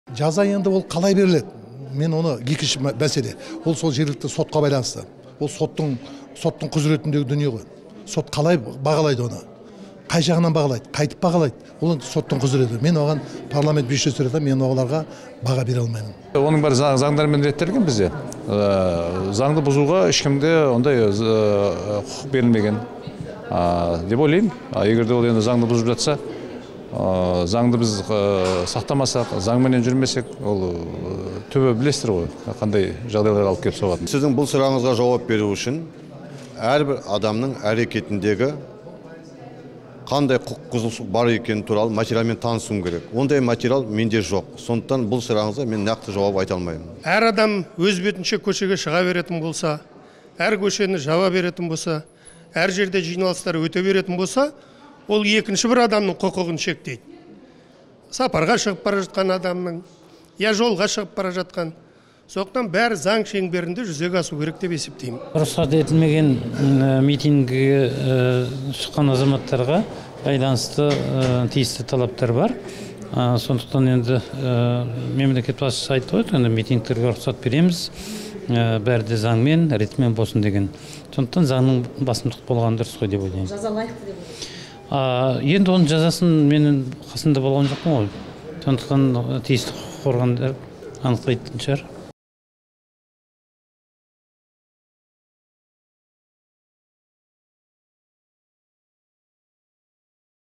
Наразылыққа шыққандарды қамау дұрыс па?
Өткен аптада Қазақстанда ондаған адам "бейбіт митинг өткізу заңын бұзды" деген айыппен жауапқа тартылған. Олардың арасында 10-15 тәулікке қамалғандар бар. Азаттық тілшісі сенат депуттарынан "Билік рұқсат бермеді деп, митингілерге шыққандарды қамау, жауапқа тарту қаншалықты орынды " деп сұраған.